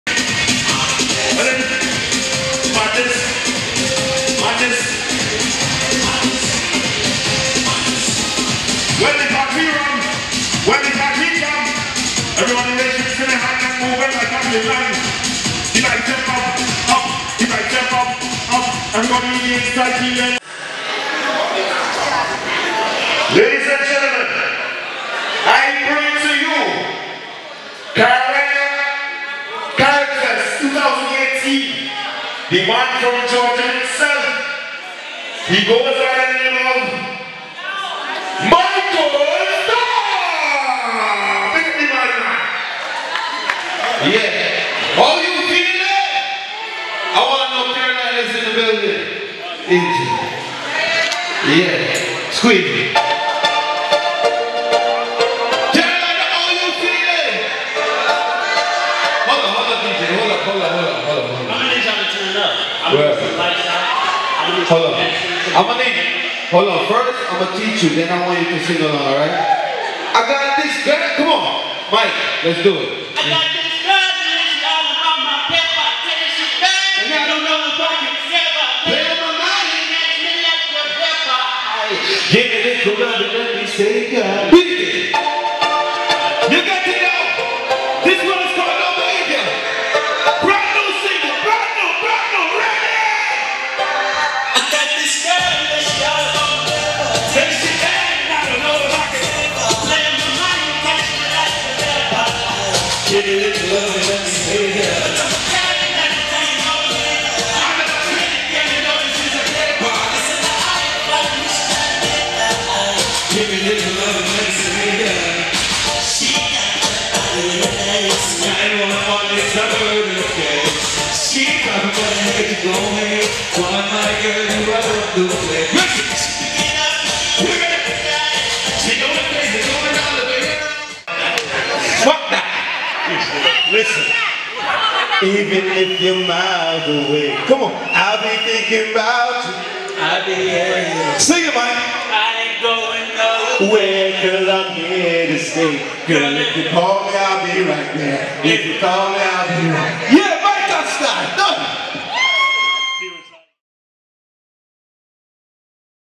Michael Star performance snippet
Short performance snippet of Michael Star at Columbia, SC 1st Caribfest 2018.

Shot & Edited by